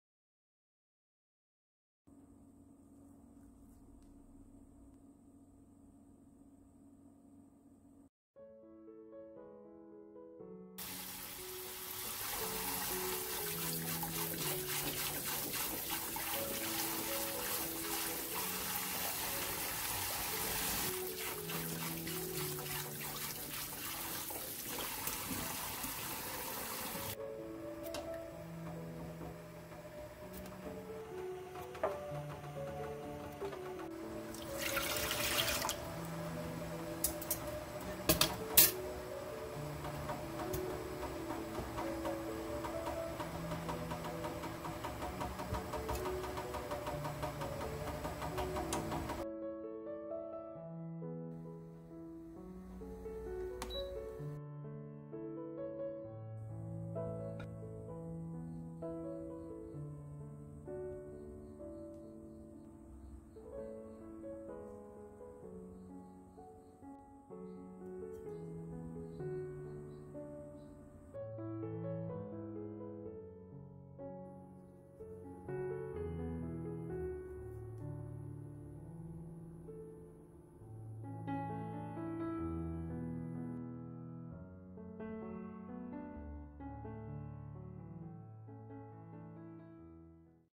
🍠고구마삶는법 불조정 탈 염려 없이 초간단 고구마 맛있게 삶는법
고구마삶는법

준비
고구마 (6개)
(밥솥에 들어갈 양)
물 300ml
찜기
전기압력밥솥

고구마 삶는 시간
만능찜 기능 30분 취사하세요 끝~